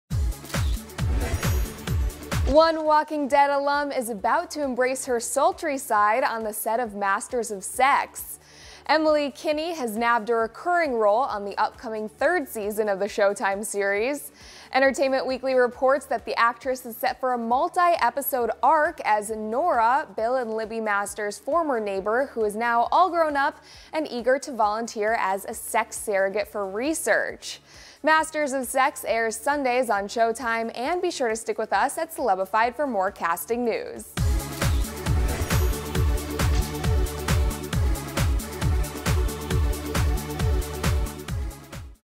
Emily Kinney is All Grown Up on Masters of Sex! The Walking Dead Alum Joins Showtime Drama
Emily Kinney has fled from zombies on The Walking Dead, tapped into her villainous side on The Flash, and now she's about go to sultry on the set of Masters of Sex!

One Walking Dead alum is about to embrace her sultry side on the set of Masters of Sex. Emily Kinney has nabbed a recurring role on the upcoming third season of the Showtime series. Entertainment Weekly reports that the actress is set for a multi-episode arc as Nora, Bill and Libby Masters' former neighbor who is now all grown up and eager to volunteer as a sex surrogate for research. Masters of Sex airs Sundays on Showtime, and be sure to stick with us at Celebified for more casting news.